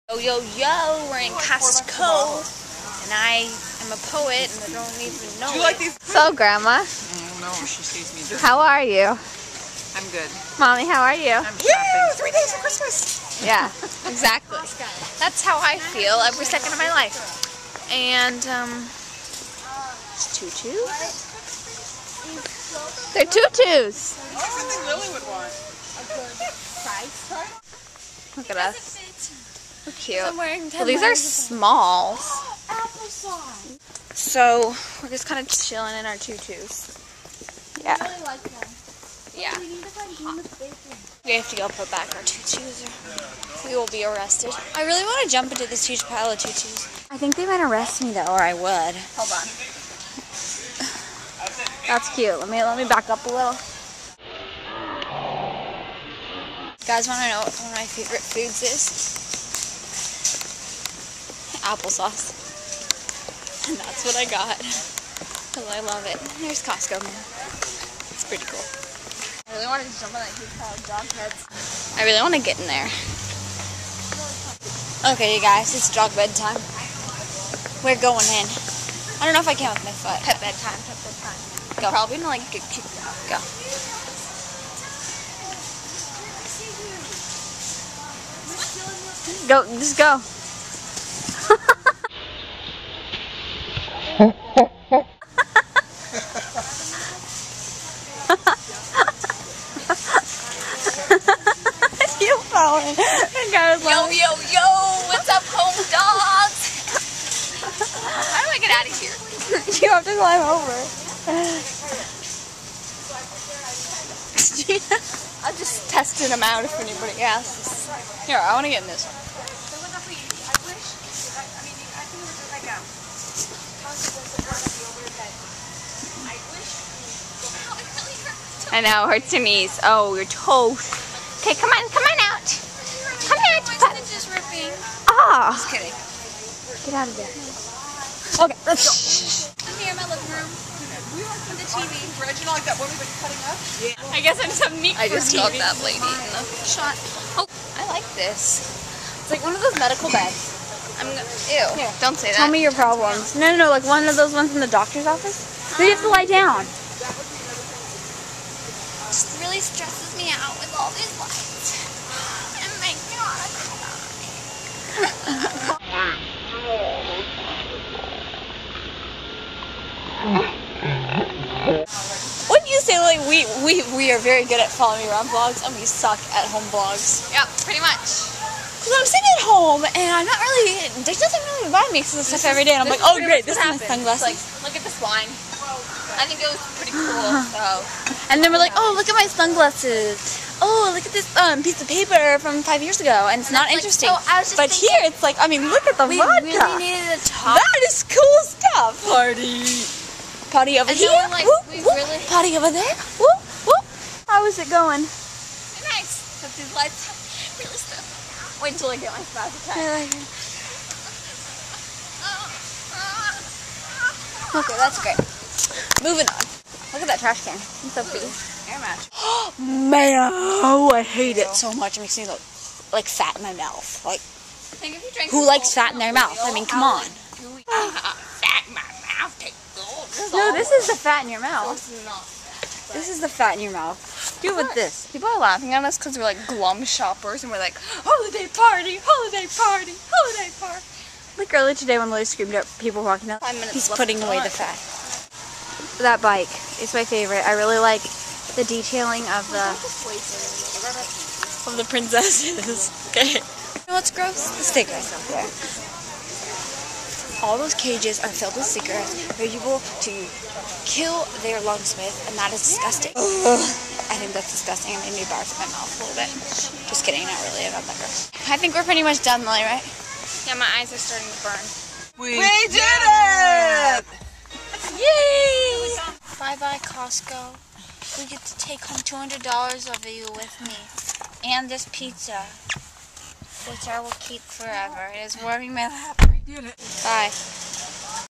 Who likes fat in their mouth?
Follow me around vlog in costco.  Filmed about a week ago :)
Julia and Lily